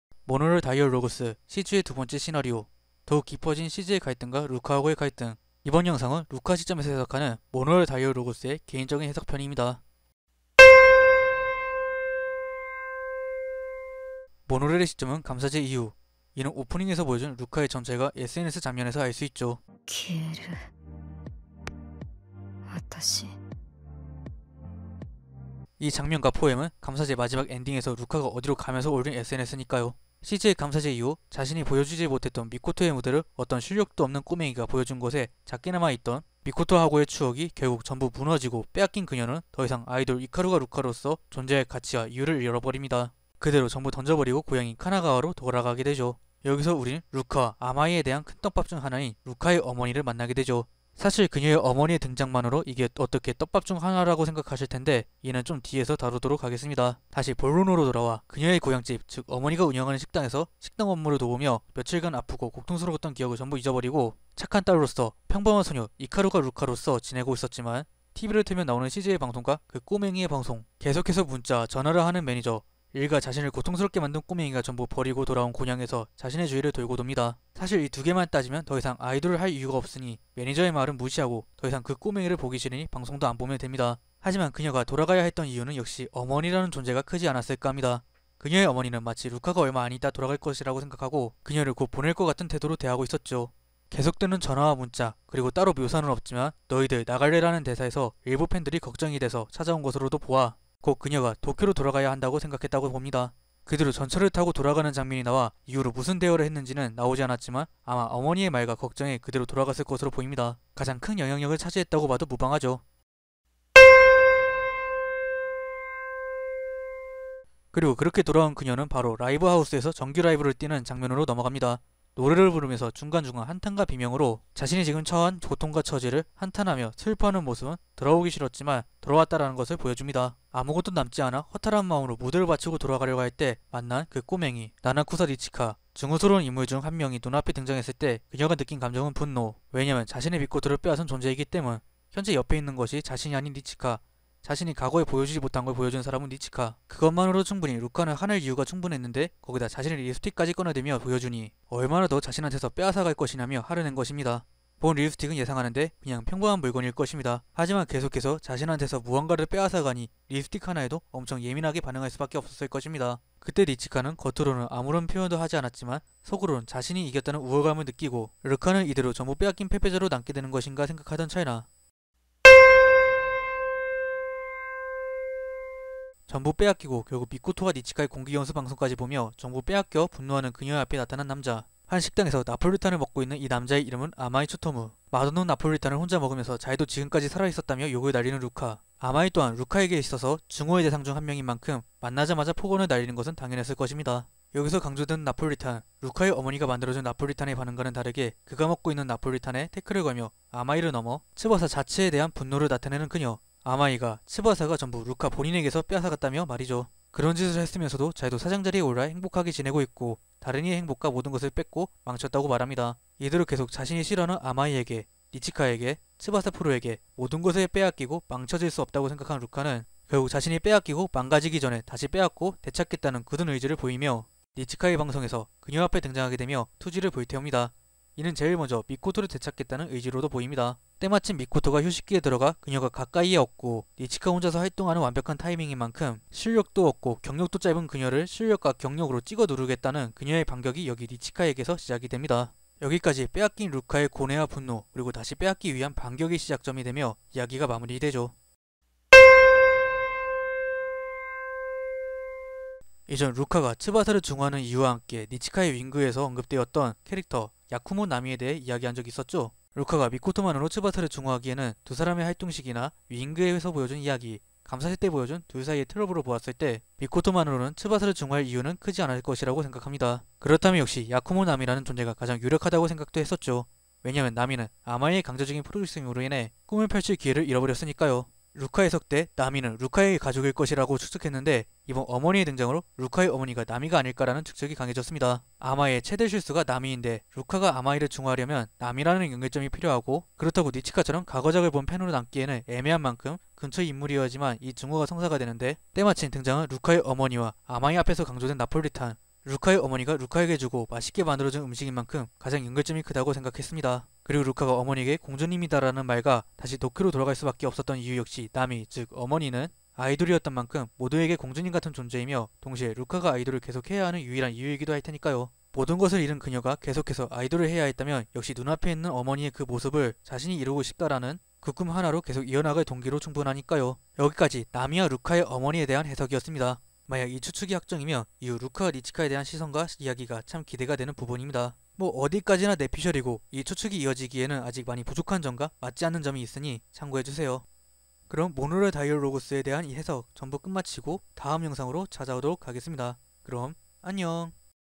계속 뺏겨 온 신의 반격 [모노럴 다이얼로그스 루카 시점 감상평]
거 립스틱 가지고 쨰쨰하시네요 카미사마

00:00 인트로
00:10 아픈그녀
01:53 빼앗긴 그녀
02:52 빼앗은 자들
04:09 나미와 어머니
05:38 마무리